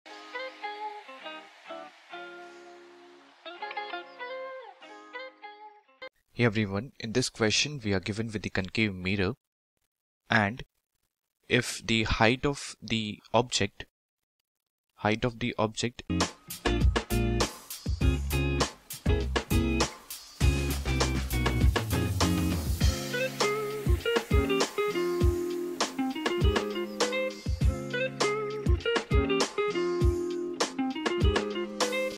4. A concave mirror produces 10 cm long image of an object of height of 2cm. What isthe magnificati…
4. A concave mirror produces 10 cm long image of an object of height of 2cm. What isthe magnification produced